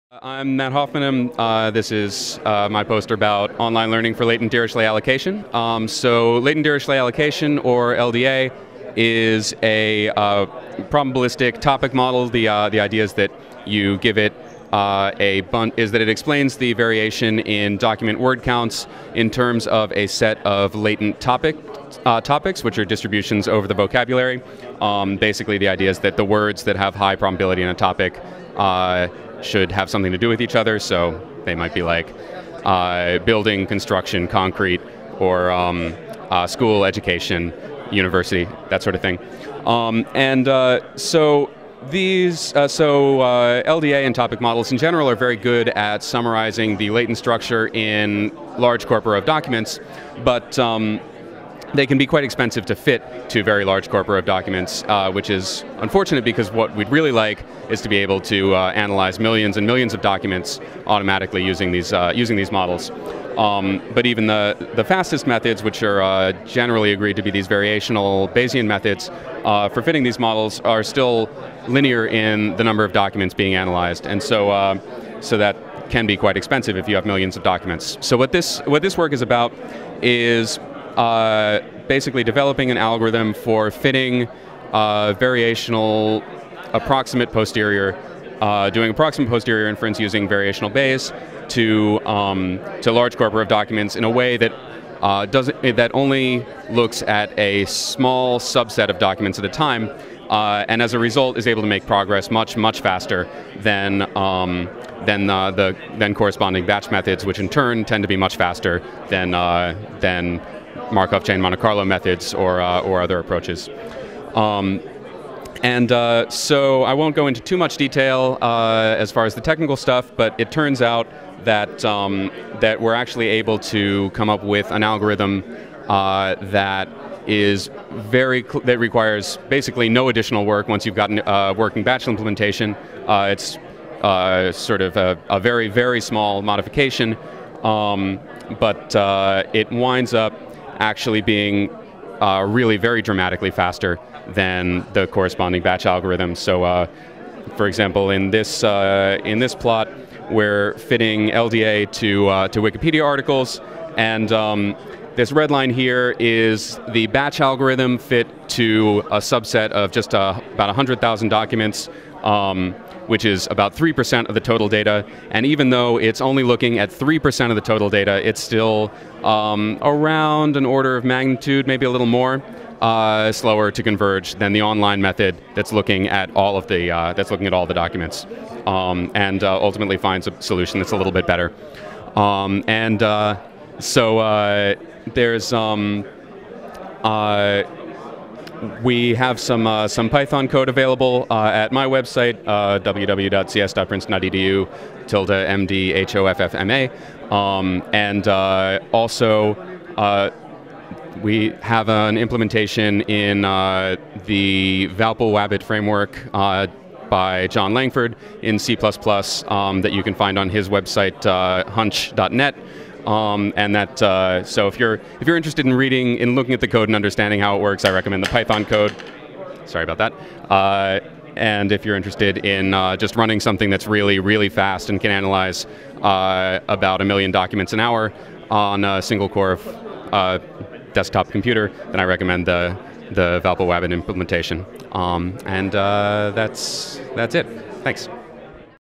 ITA Poster Session 2011 Matthew Hoffman
Matthew Hoffman - Online Learning for Latent Dirichlet Allocation